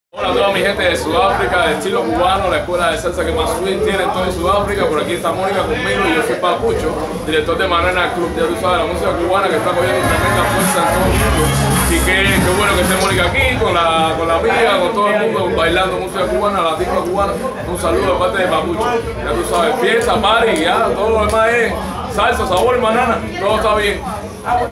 Greetings from Papucho of Manana Club
Greetings from Papucho (Director of Manana Club) to all Timberos & Casineros in South Africa directly from El Diablo Tun Tun in Havana!